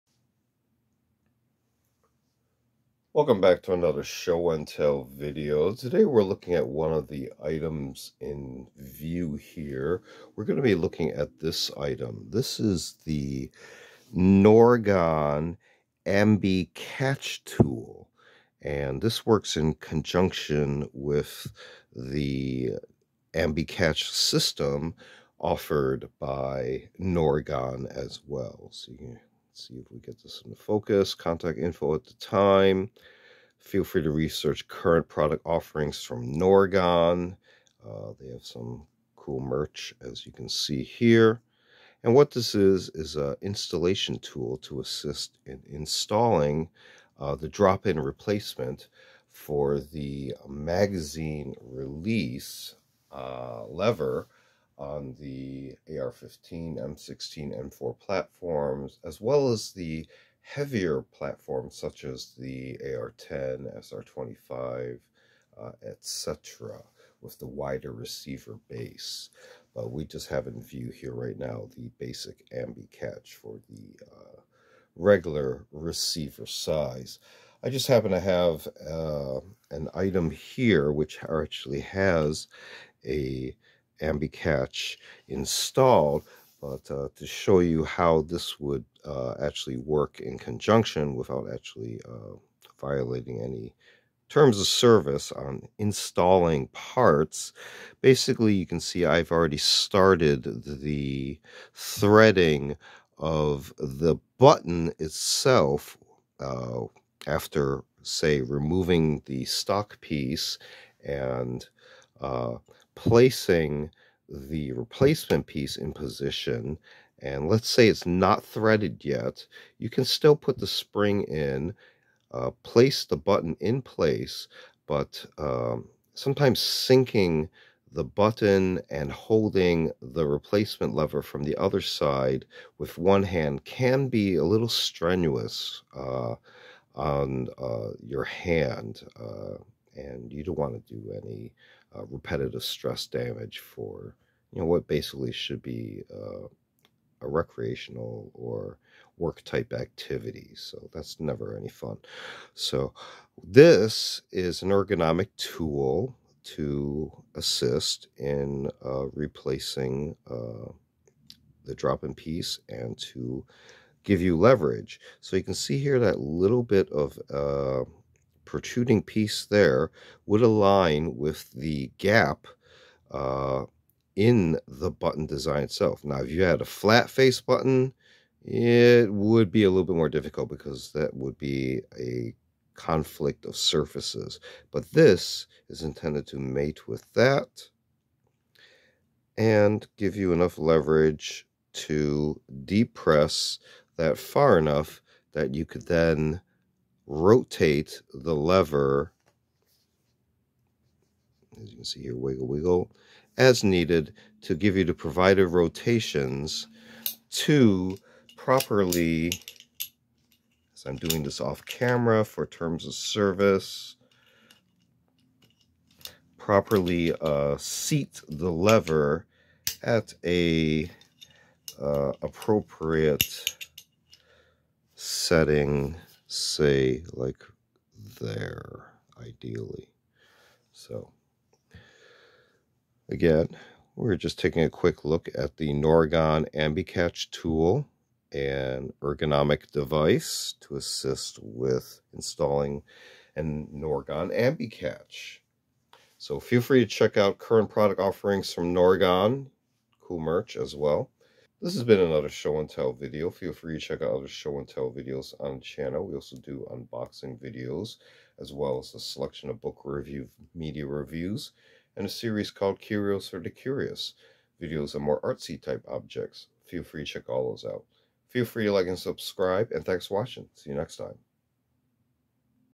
SHOW & TELL 117: NORGON, LLC Ambi- Catch Tool. M16, AR15 (M4) Ambi Mag Release Installation Tool.
SHOW AND TELL [117]: NORGON, LLC Ambi- Catch Tool.  Ambidextrous Magazine Release Mechanism Installation Tool.  M-16, AR-15, M-4, SR-25, AR-10 etcetera.